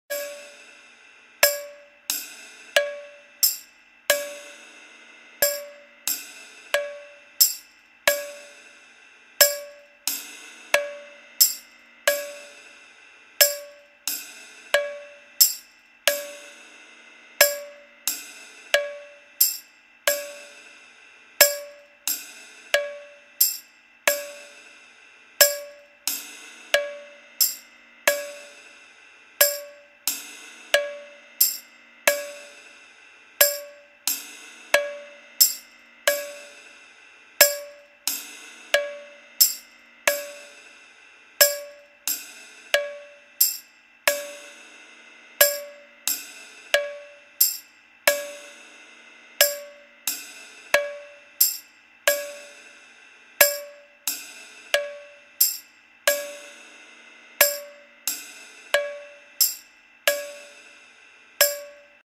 Two rhythms ♩ ♪ and ♩ combined as a polymeter - with Bounce Metronome Pro
One of the rhythms in Bounce 4.3 - ♩ ♪ in one part, ♩ in the other part